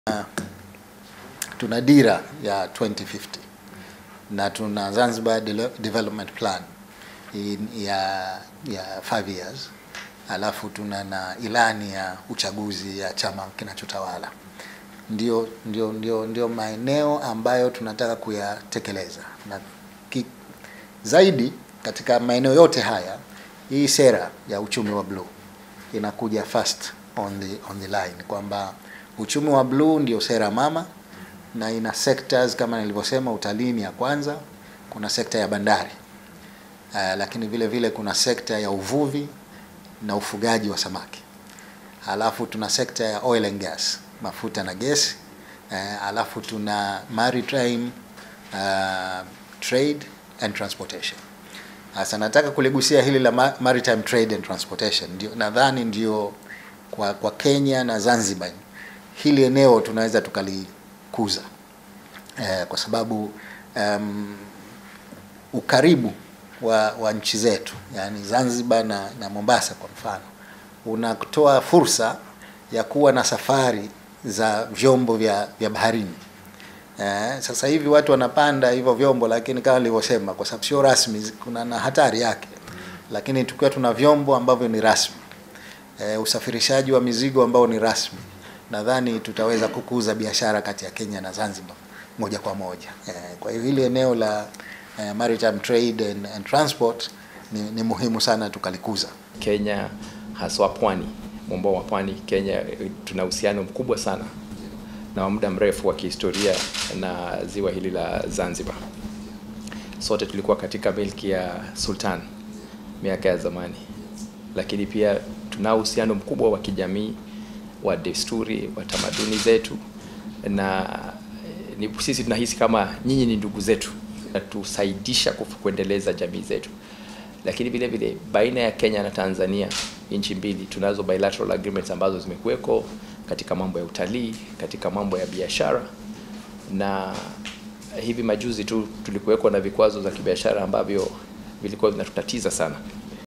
Balozi wa Kenya atinga ikulu Zanzibar na kuzungumza na Rais Mwinyi
Video from Ali Kombo